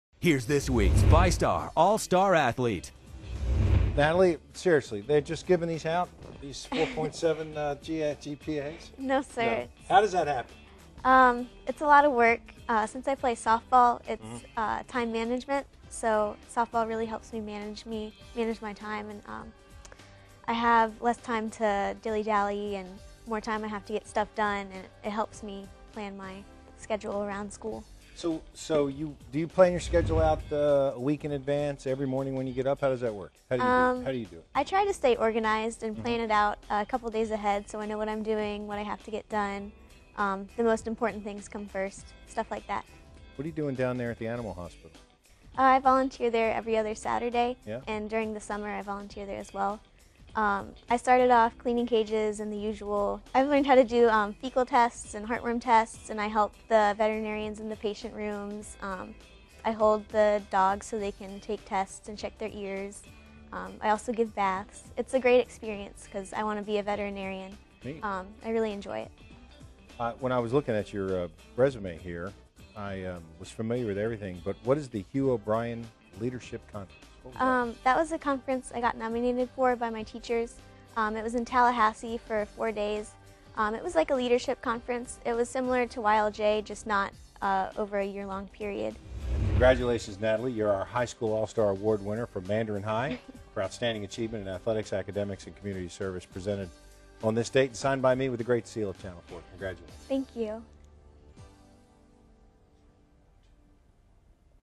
VyStar All-Star Athlete Natalie Cleghorn
VyStar All-Star Athlete Natalie Cleghorn is a student at Mandarin High School. She plays Softball and Volleyball as well as maintains a 4.7 GPA.